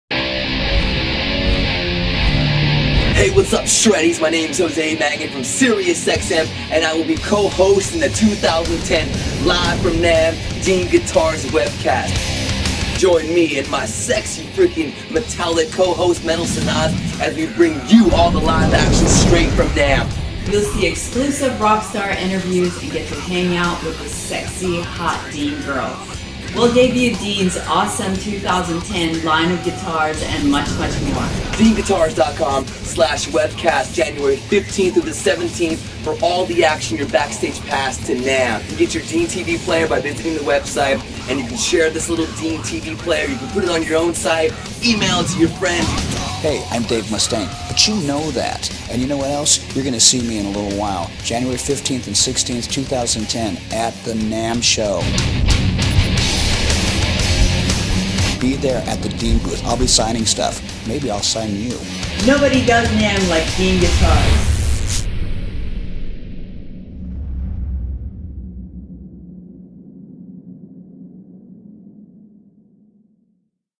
Dean Guitars "Live From NAMM 2010" Promo! New Video!
Jose Mangin and Metal Sanaz host Dean's 4th Annual "LIVE FROM NAMM" Webcast! Dean Girls, exclusive artist interviews, new product spotlights, and more!!

Over 350,000 viewers last year! starting Friday Jan. 15, 2010 at 10am!